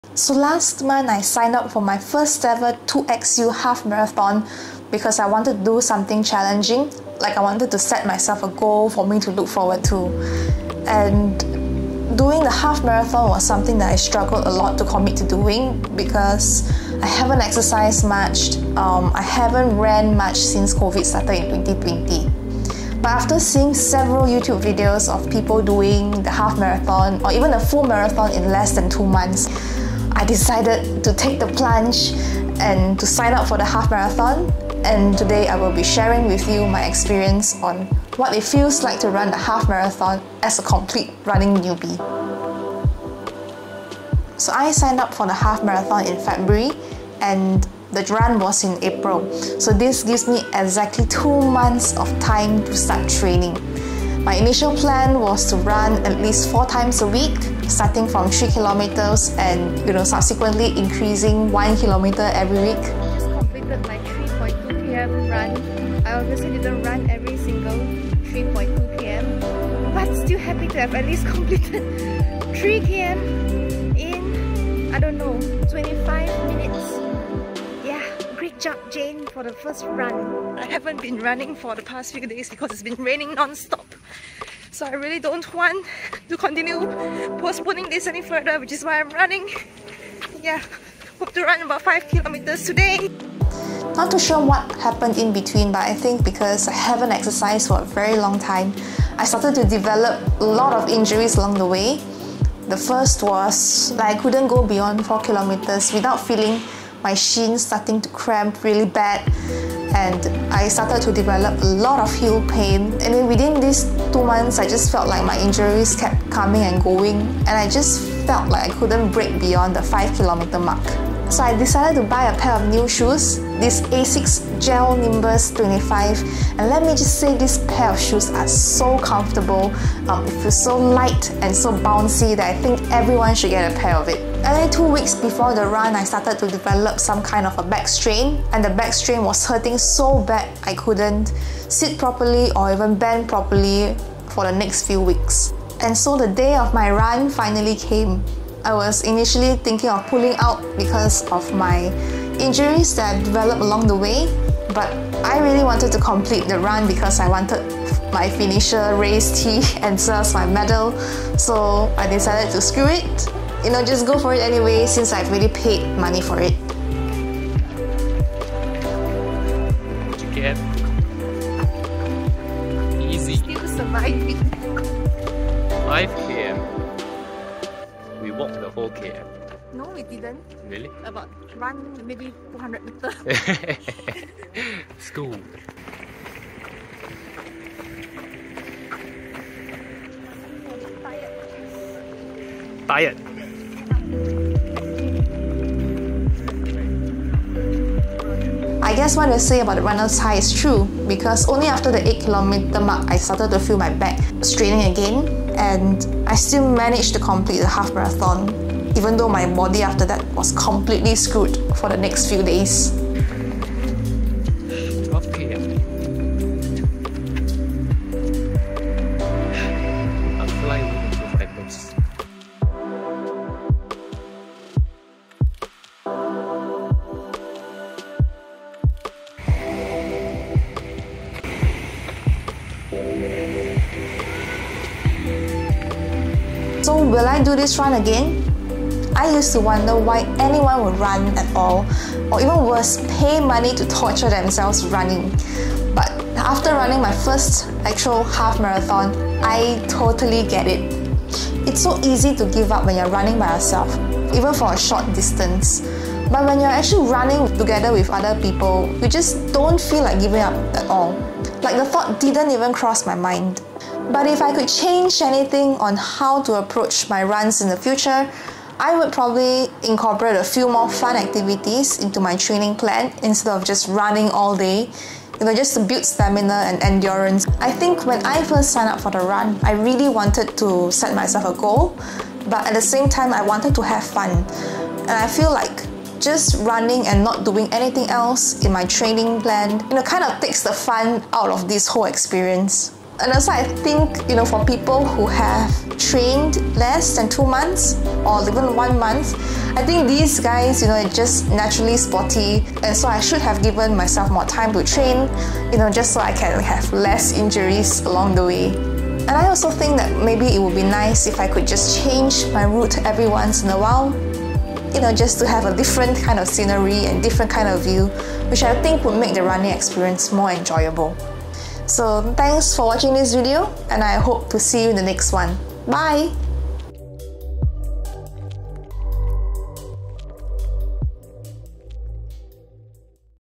Running a half marathon without much training
How I ran a half marathon without much training due to injuries sustained along the way. Will absolutely run another half marathon again, next time with proper training!